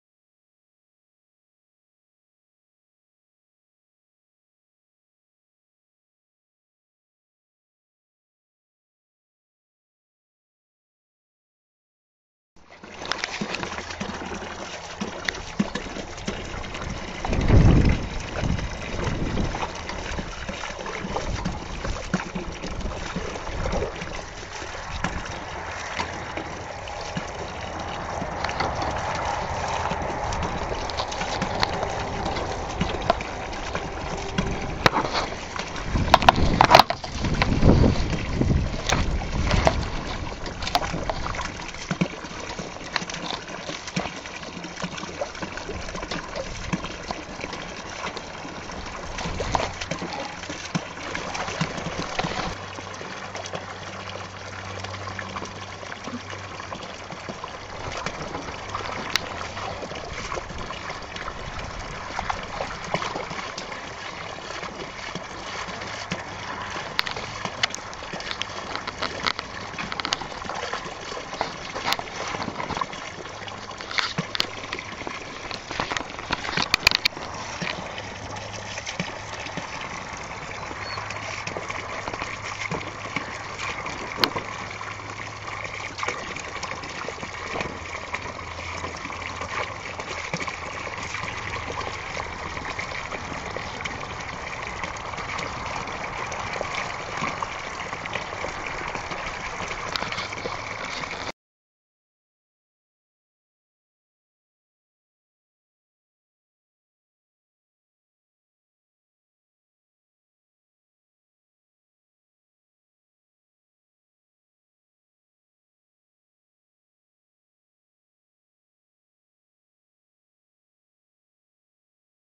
schnauzer-chiot 5 mois et l'eau .wmv
Chiots schnauzer moyens de 5 mois qui jouent dans l'eau.